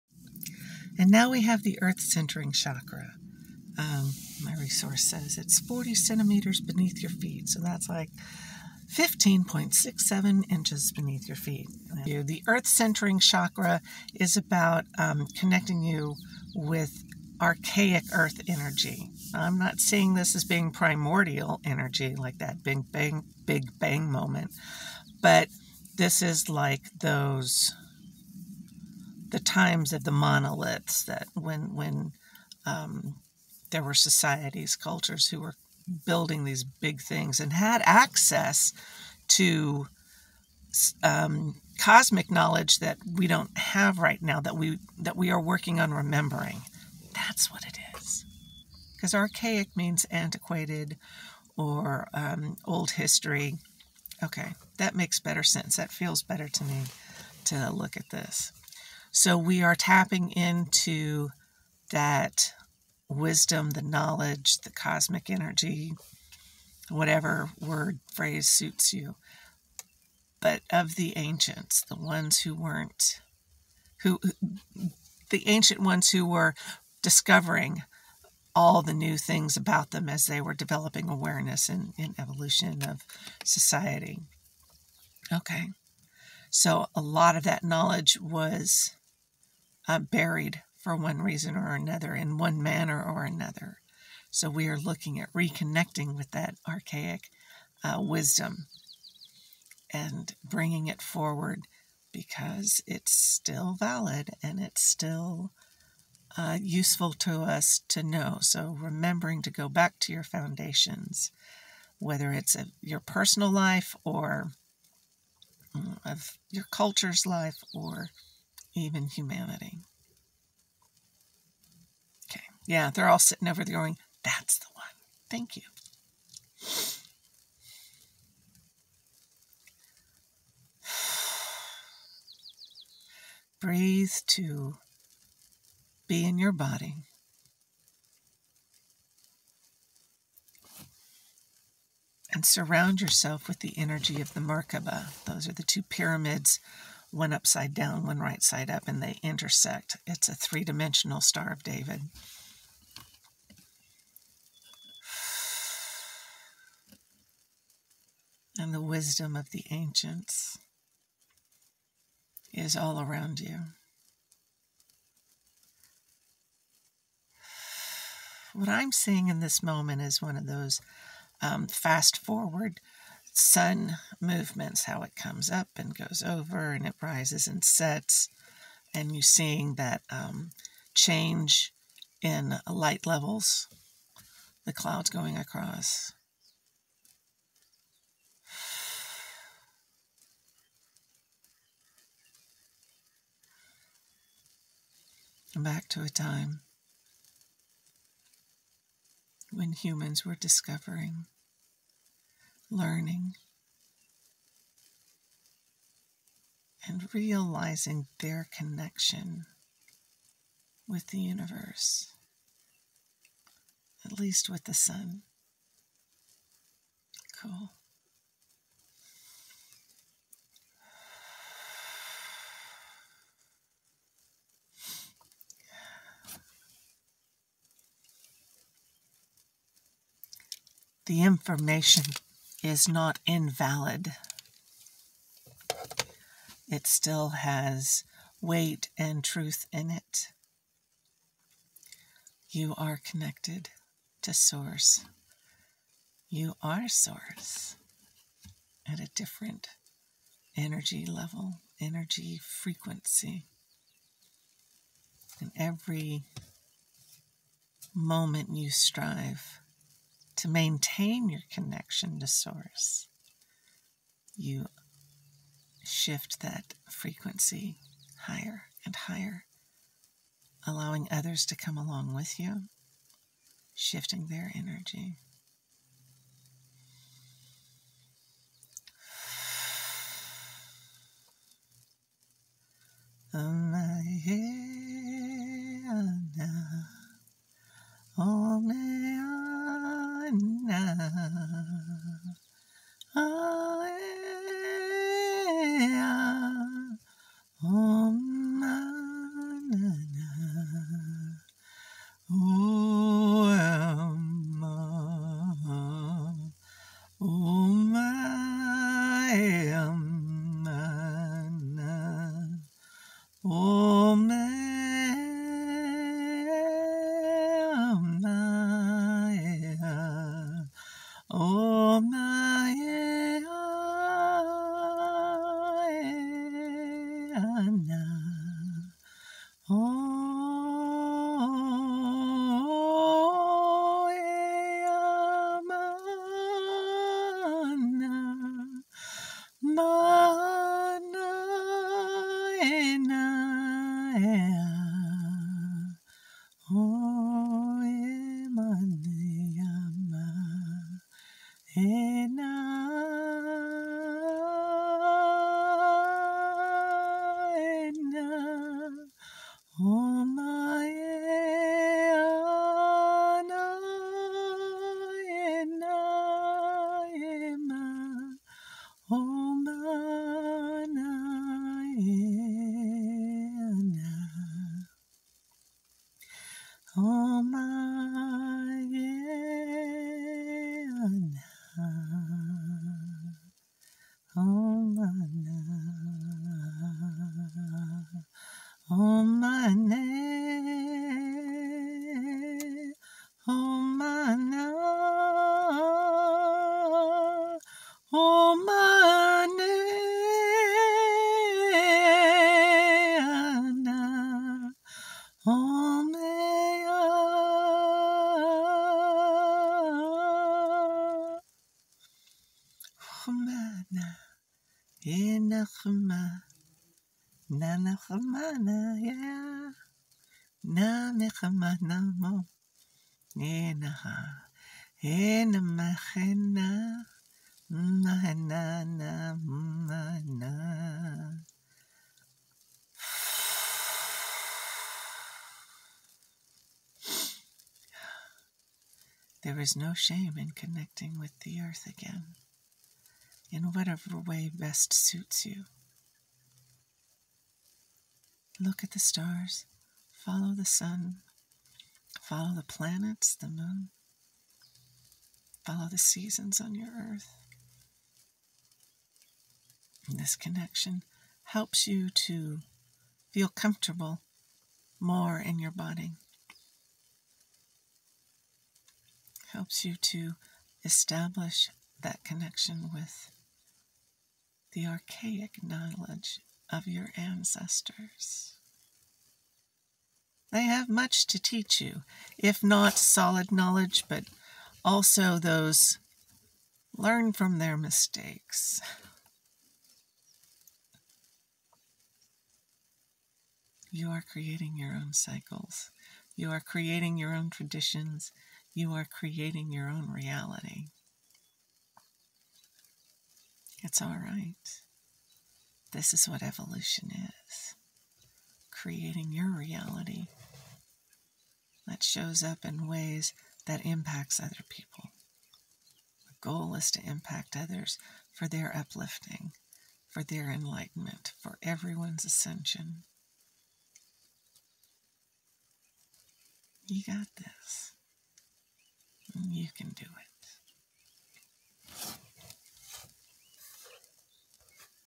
Light Language - Chakras - Earth Centering
The Earth Centering Chakra connects you with ancient earth wisdom. Think monoliths and henge building cultures.